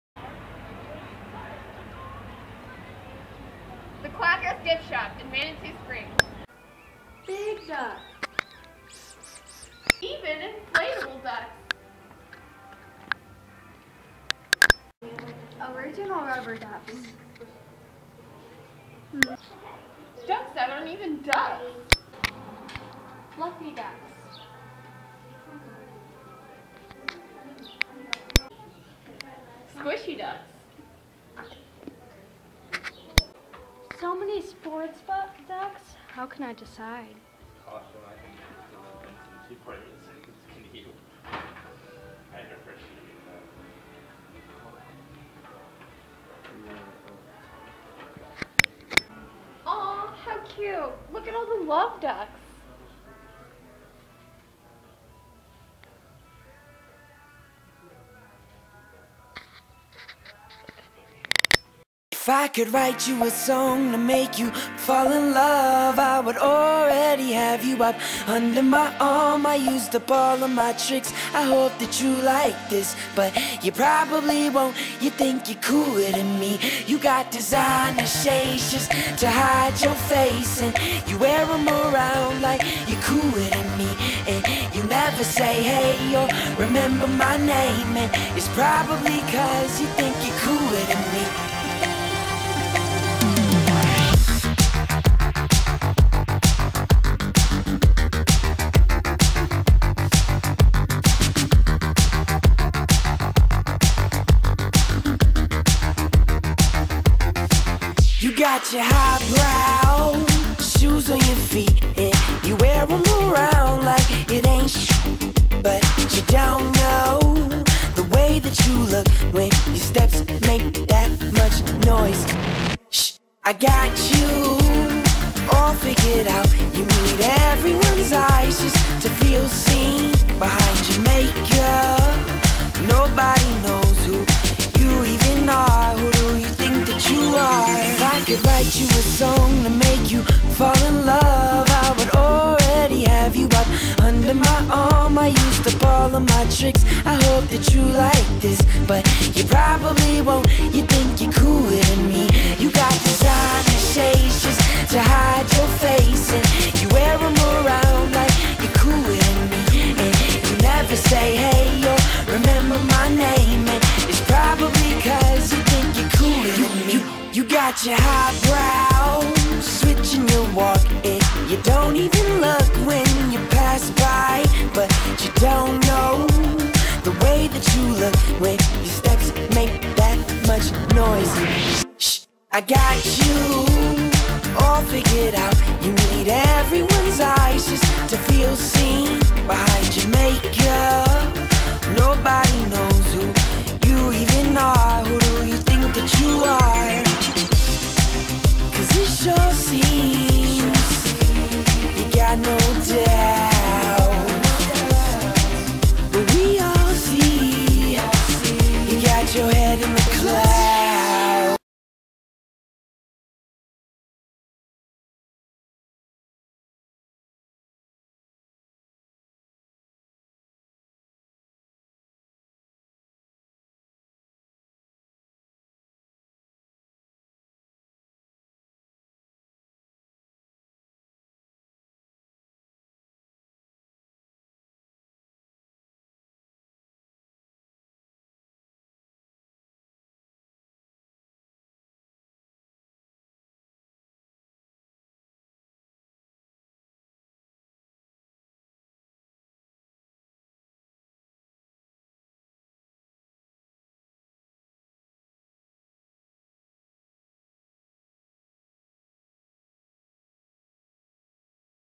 Madi and Baylee make their first Quacker video!
Baylee and Madi are two young ladies who on summer vacation in Manitou Springs have befriended our Quacky little store.  This is their first of many installments this summer.  Be sure to look for the next video from Madi and Baylee's great duckie adventure